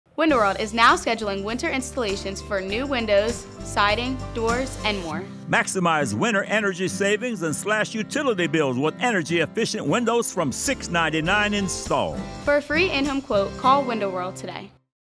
Window World of Philadelphia | ❄️ Philadelphia’s Winter-Ready Home Sale!
Warm up to savings at Window World of Philadelphia
As the temperatures drop, it's a good time to upgrade your home with beautiful, energy-efficient windows, doors, and siding from Window World of Philadelphia! Each come with a lifetime guarantee and, for a limited time, no interest financing is available for those who qualify.